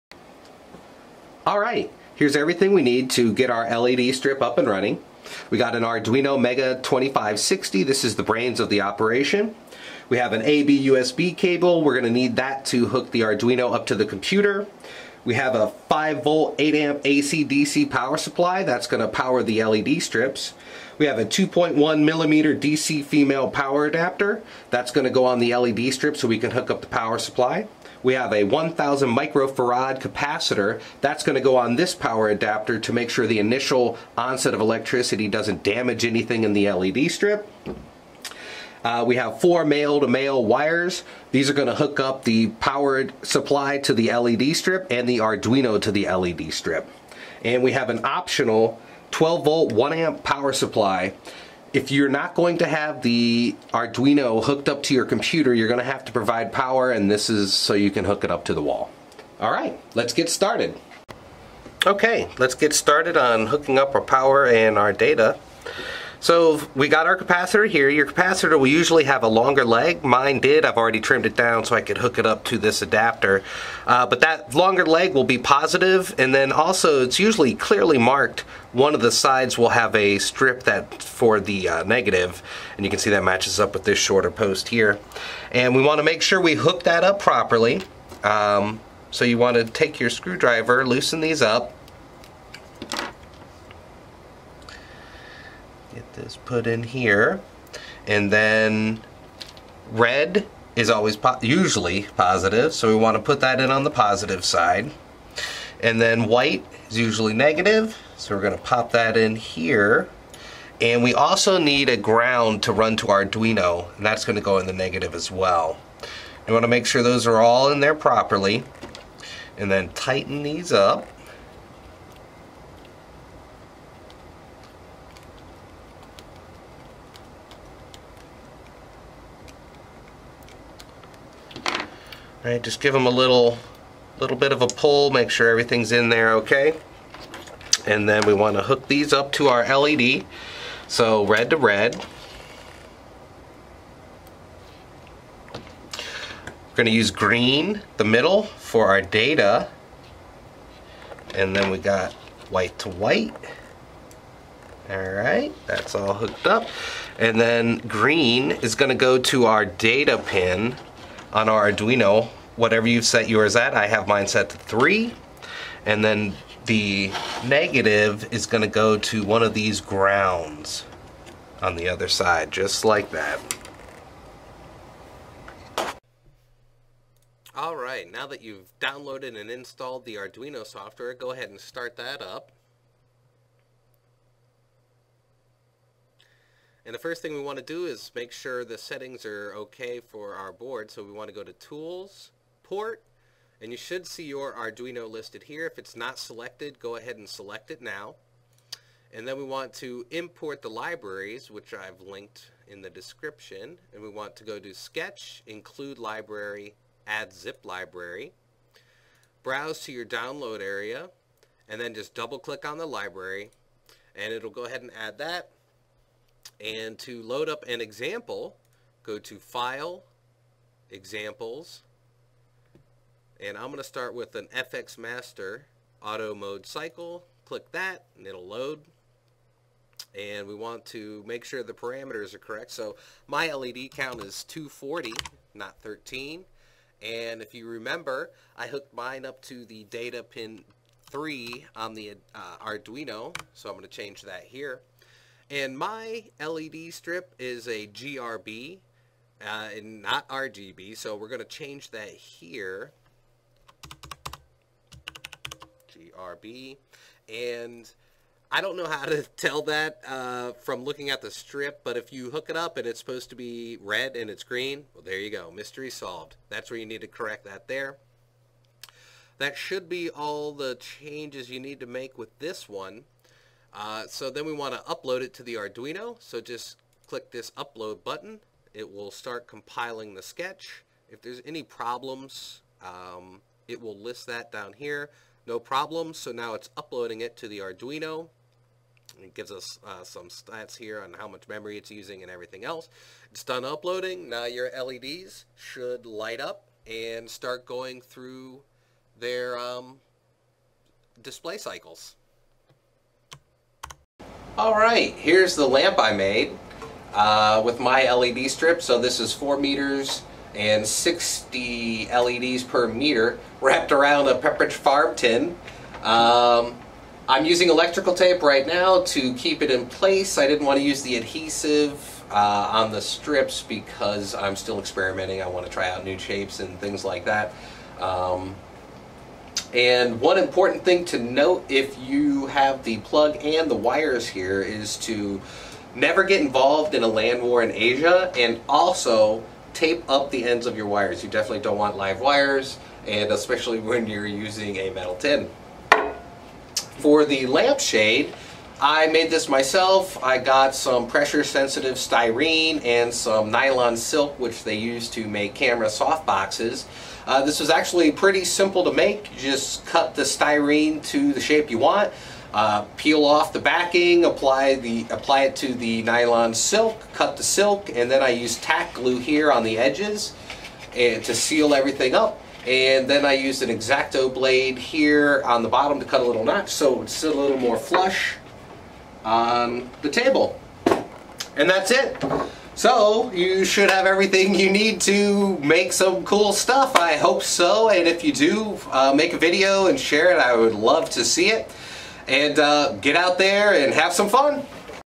Quick tutorial on getting a WS2812 strip setup with an Ardruino 2560
List of hardware
Arduino Mega 2560
A/B USB cable
2.1mm DC famale power adapter
5v 8a AC/DC power supply
1000uf capacitor
4 male-to-male wires
12v 1a power supply